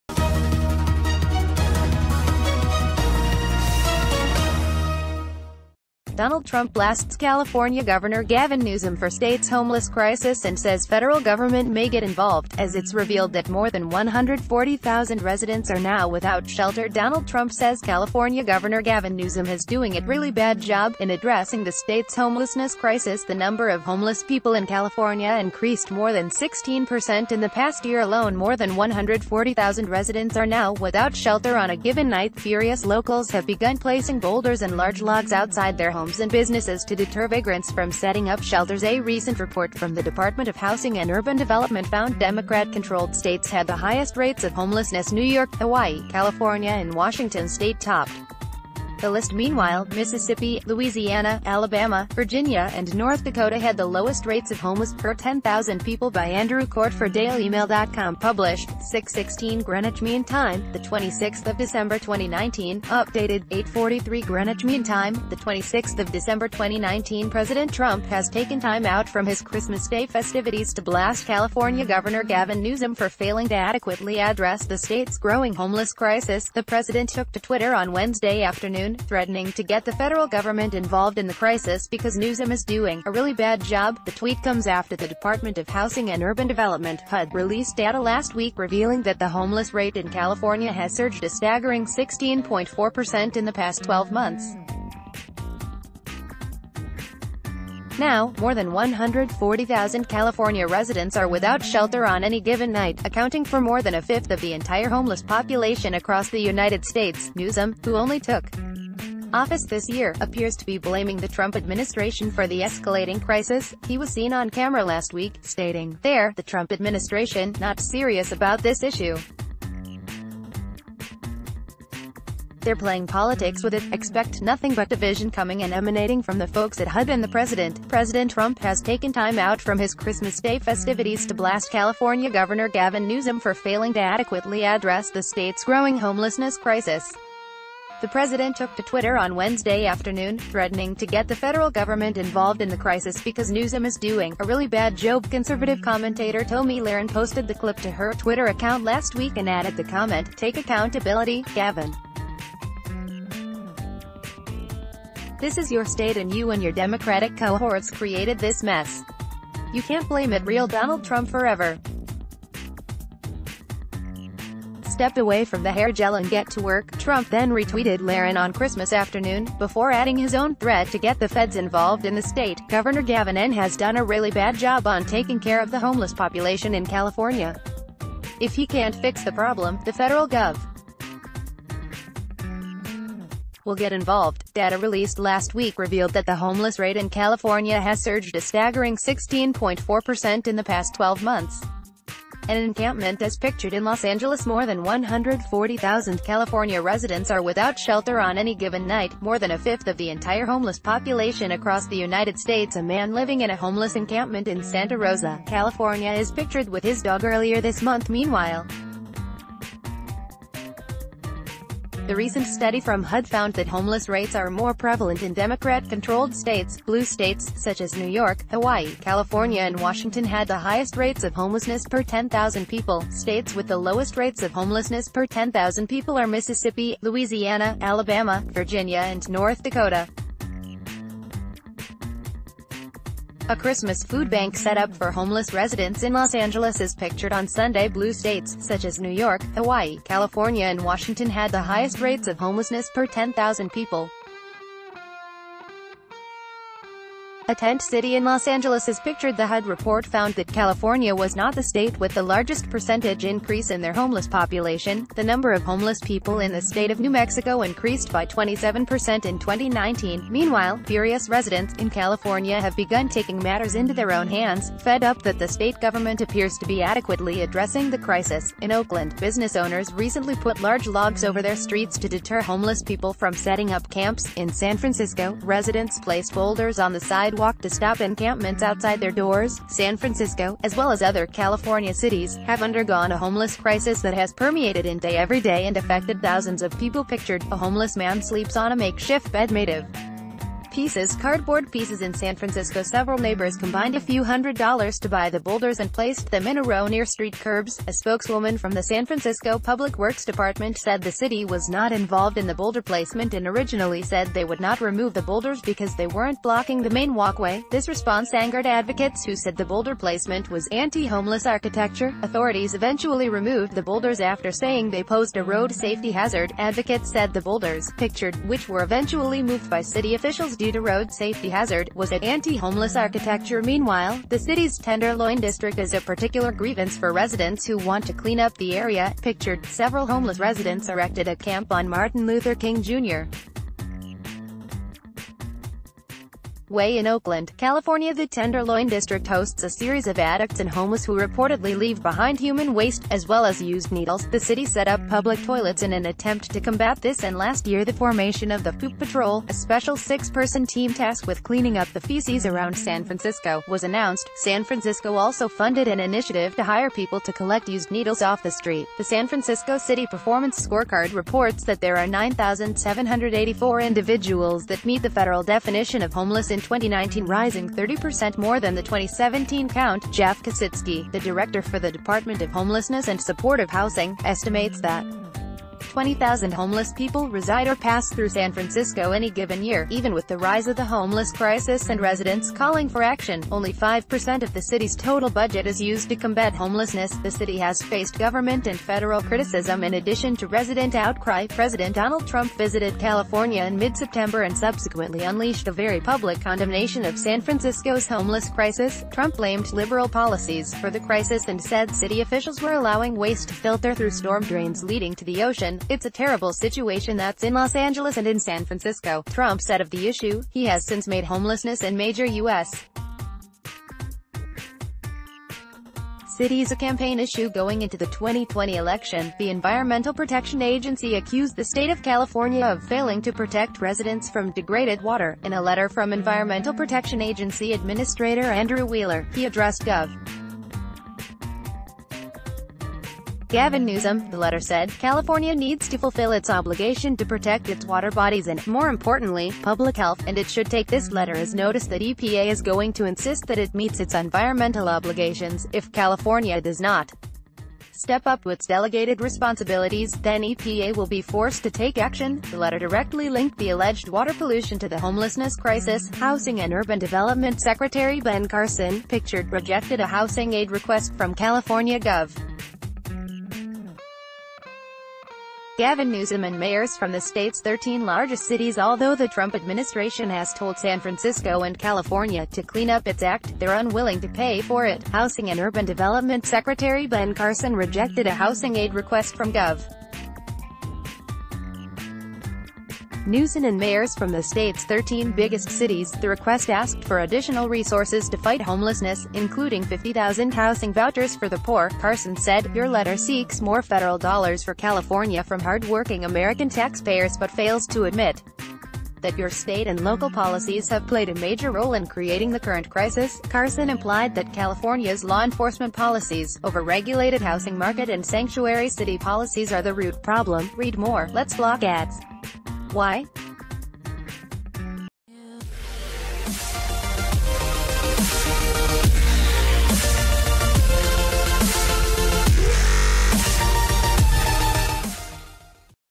Trump blasts California Governor Gavin Newsom for homelessness crisis
Donald Trump blasts California Governor Gavin Newsom for state's homeless crisis and says federal government may get involved - as it's revealed that more than 140,000 residents are now without shelterDonald Trump says California Governor Gavin Newso...